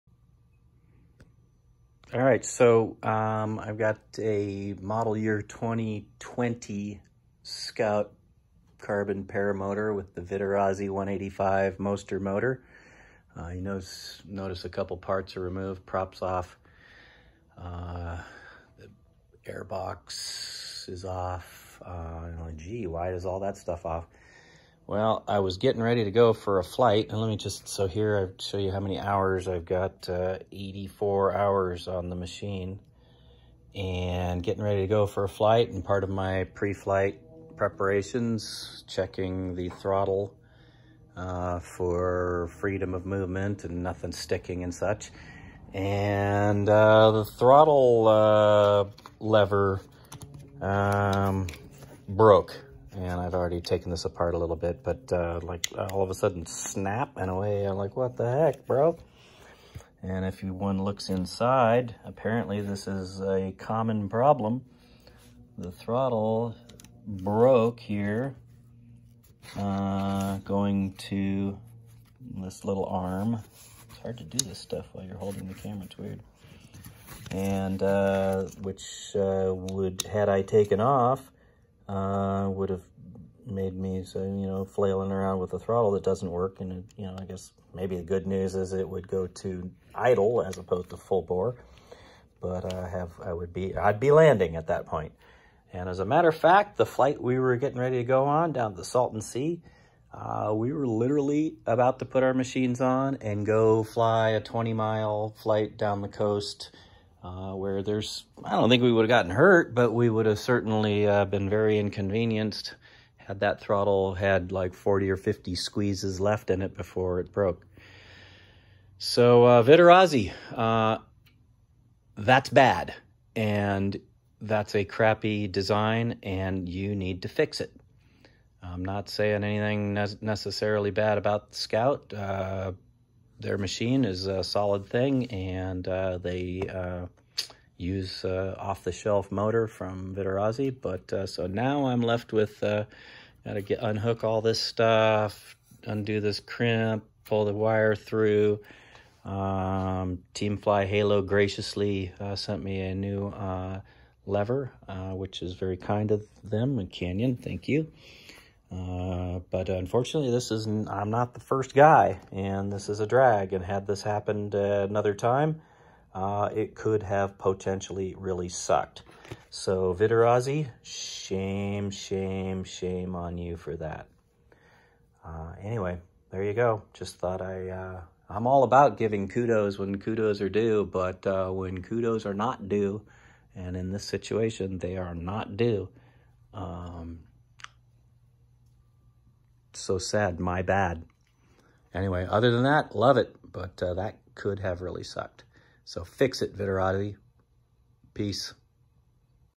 Vittorazi Moster 185 Paramotor Throttle Broken
Getting ready to fly, I was doing preflight inspection and the throttle lever broke in my hand!  Fortunately this was BEFORE I TOOK OFF!  Apparently this is a fairly common occurrence.  In my opinion this is a crappy design and needs to be addressed by the manufacturer.